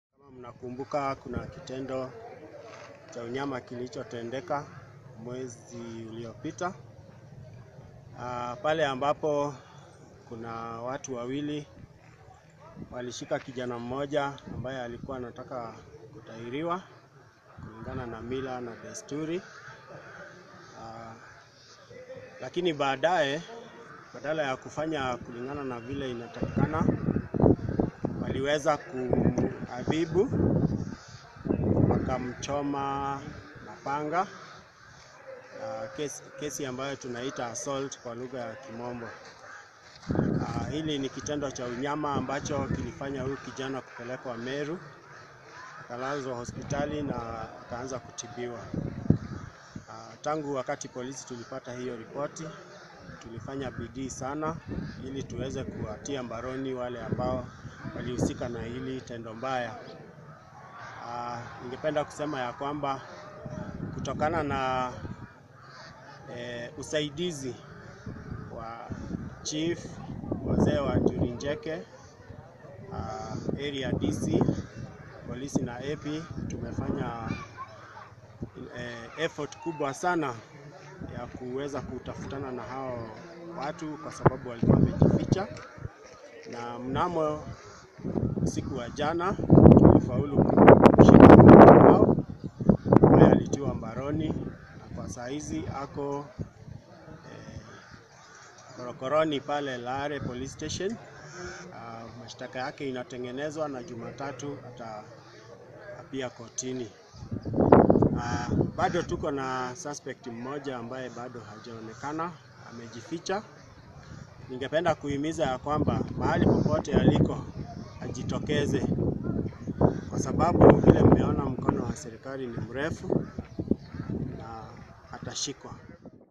One of the suspects who burnt a boy with the hot panga arrested
Police in Igembe North have arrested one of the suspects who burnt a boy with a hot panga after entering initiates’ seclusion house. Seventeen-year-old Benisa Michubu, who is still recovering at the Meru Teaching and Referral Hospital was attacked after he entered a house in which circumcised boys were recovering last month. Area police boss Peter Kimani said the suspect, Joseph Mwiti, was arrested on Friday after a tip off from Njuri Ncheke elders.